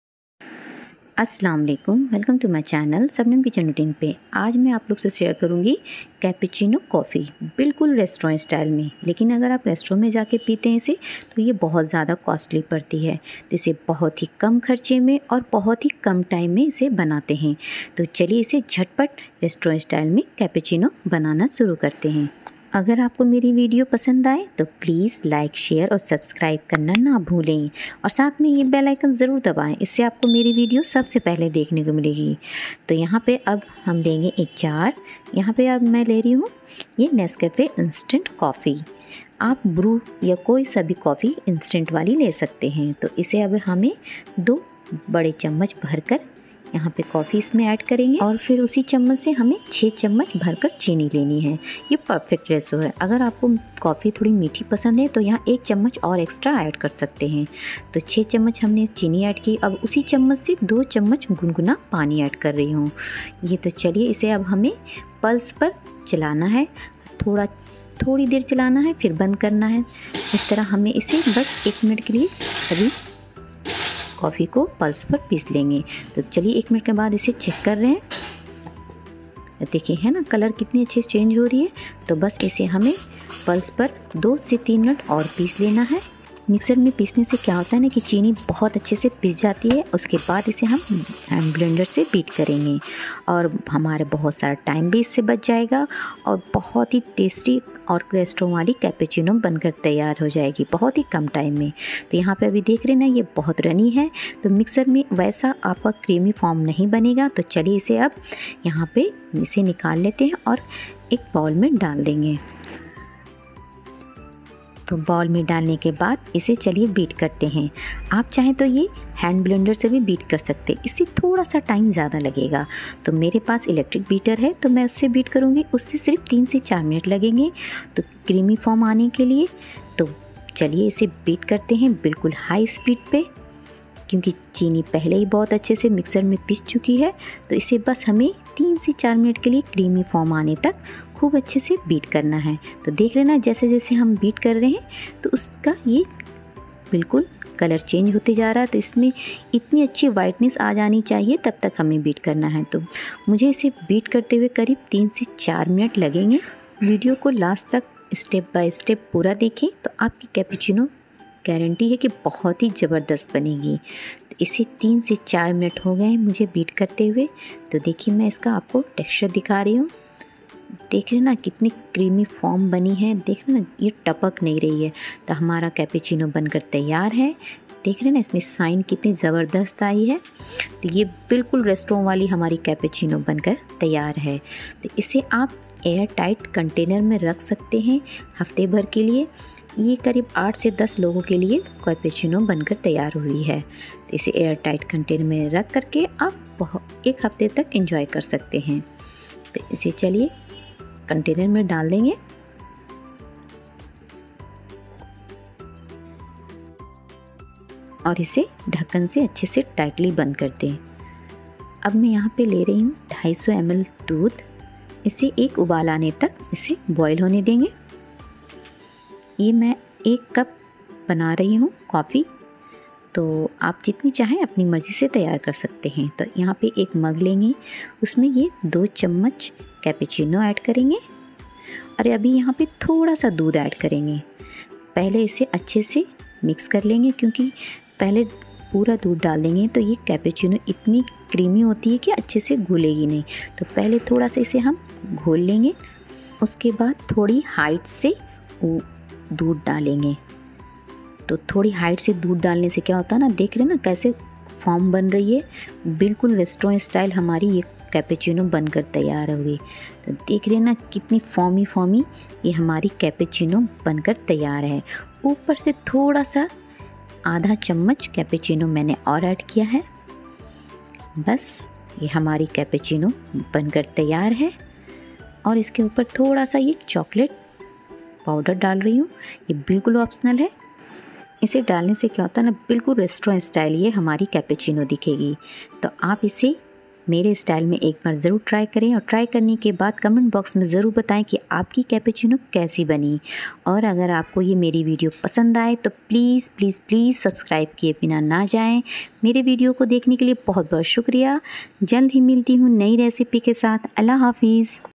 world best cappuccino coffee at home,bahut hi Kam kharche me ❤️ bahut Kam time me ghar pe hi banaye😋
World Cup best cappuccino ab Ghar per hi banana sikhe, very fast cappuccino at home, ummid hai aapko pasand ayegi 🥰